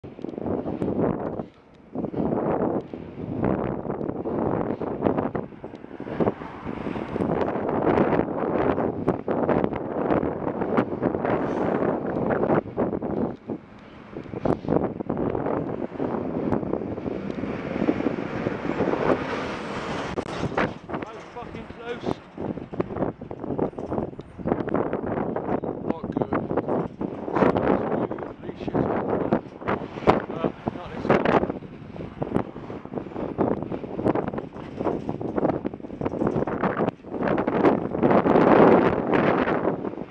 LM10XGA Pass On Brow Of Bridge
Cemex make a point of their excellent driver training regards driving around cyclists. This driver seems to have forgotten or ignored it. To pass on the brow of a bridge and then turn left at the bottom of the bridge isn't good. If the driver had stayed safely behind me then he would have been delayed by a second or two.